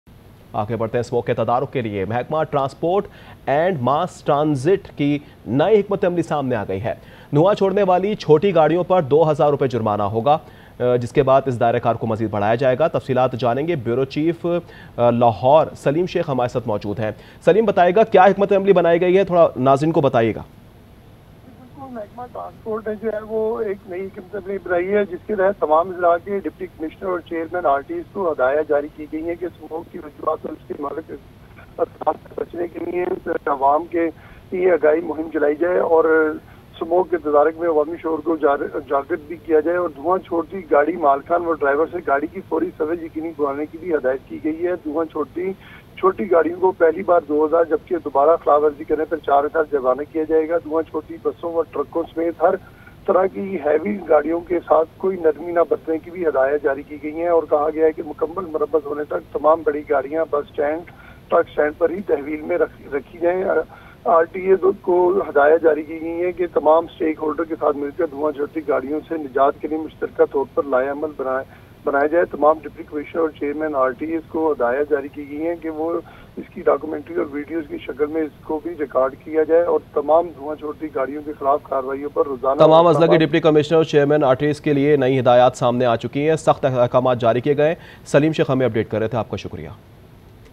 New strategy to tackle smog: Heavy fines on small vehicles - Aaj News
New strategy to tackle smog: Heavy fines on small vehicles - 12 November 2024 - Aaj News

Spot Light With Munizae Jahangir:
shorturl.at/gwFU6